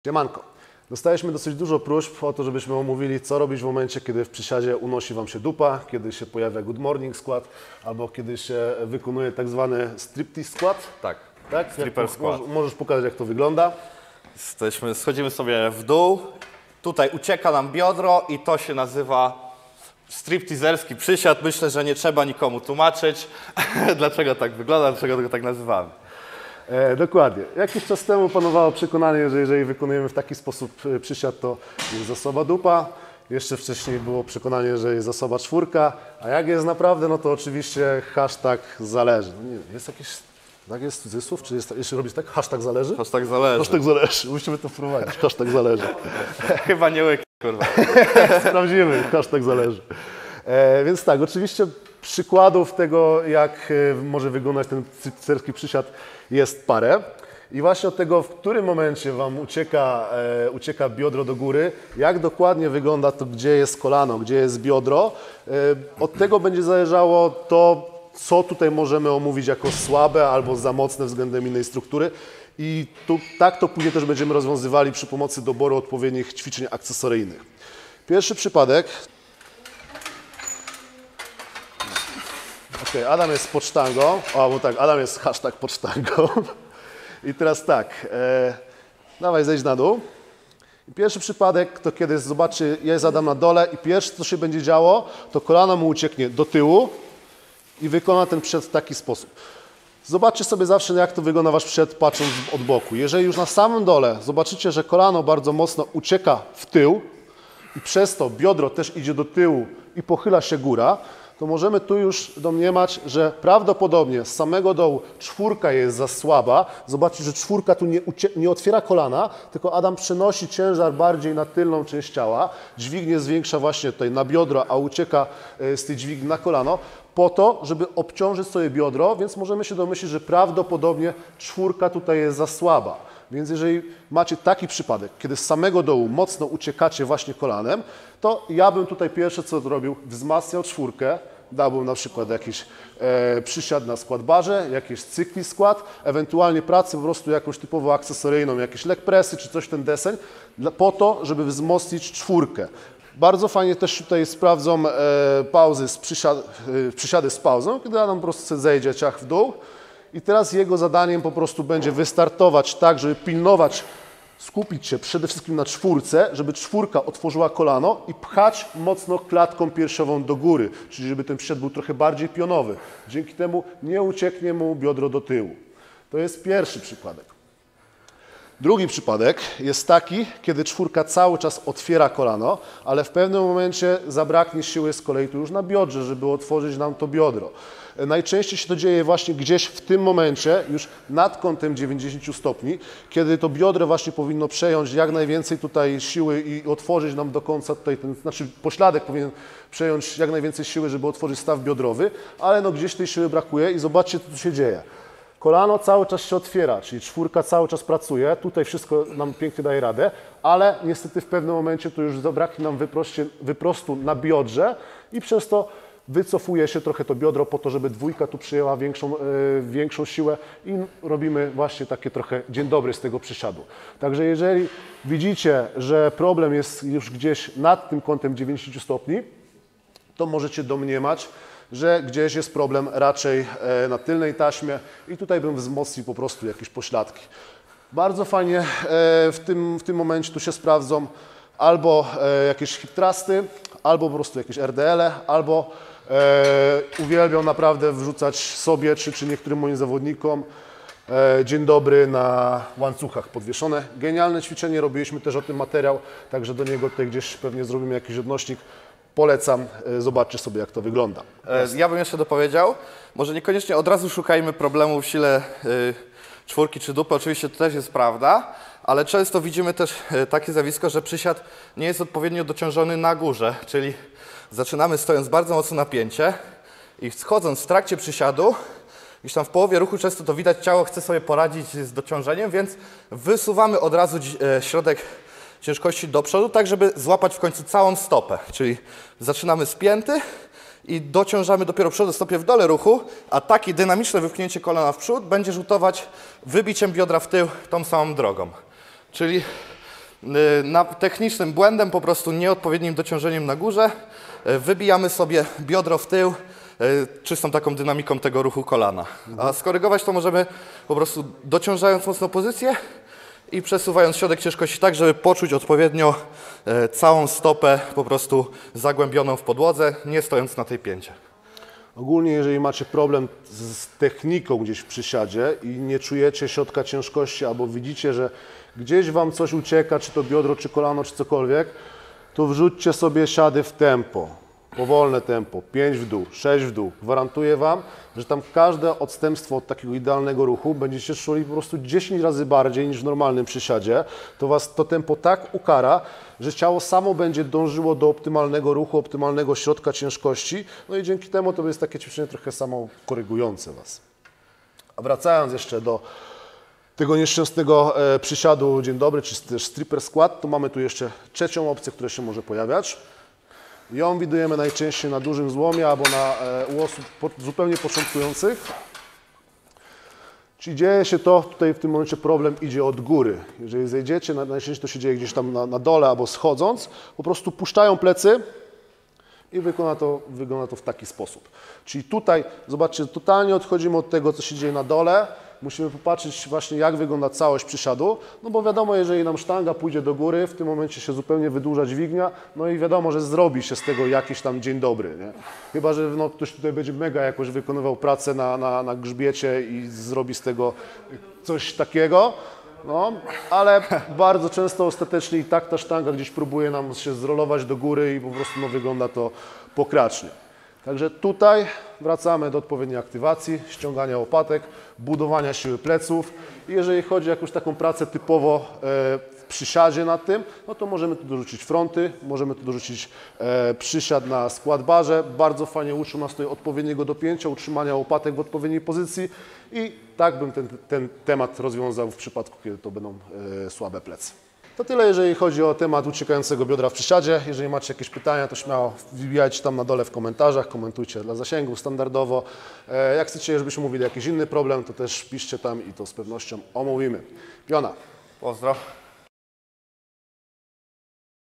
Przysiado-dzieńdobry, czyli stripper squat - jak temu zaradzić? | Podsztanga.pl
Jeżeli jesteś na kawalerskim u kumpla i przed Tobą stoi fajna laska to stripper squat może być całkiem spoko. Jednak w przypadku kiedy jesteś na siłce to zdecydowanie powinieneś go unikać - niezależnie od tego, czy podnieca on Twoich kolegów, czy nie.

Co jest przyczyną przysiado-dzieńdobry? Słaba czwórka? Słaba dupa? A może słaba psychika?
Odpal nasz materiał i zobacz jak analizujemy jeden z popularniejszych błędów w przysiadzie.